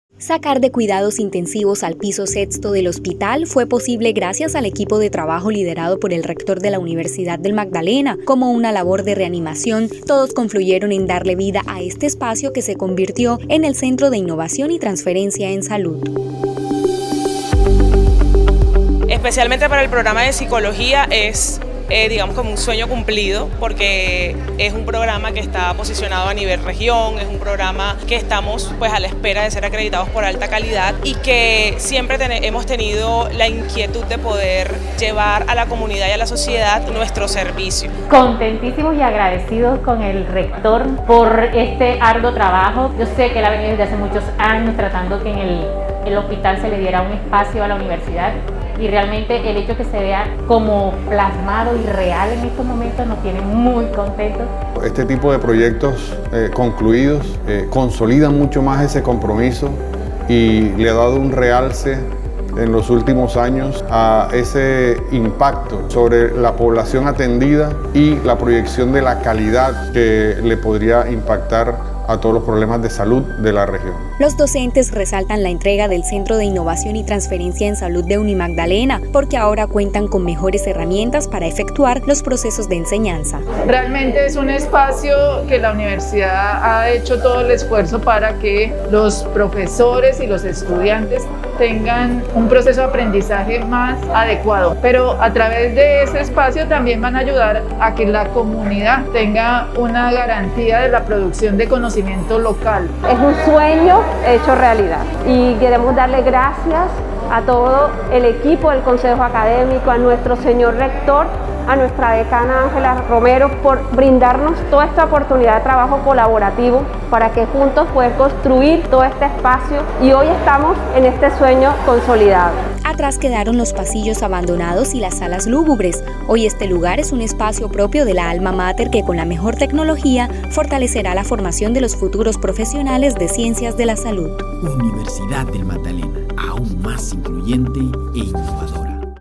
Docentes resaltan entrega del Centro de Innovación y Transferencia en Salud de UNIMAGDALENA
‘Sacar de cuidados intensivos’ al piso sexto del Hospital fue posible gracias al equipo de trabajo liderado por el Rector de la Universidad del Magdalena. Como una labor de reanimación, todos confluyeron en darle vida a este espacio que se convirtió en el Centro de Innovación y Transferencia en Salud. Los docentes de la Alma Mater resaltan esta entrega porque ahora cuentan con mejores herramientas para efectuar los procesos de enseñanza.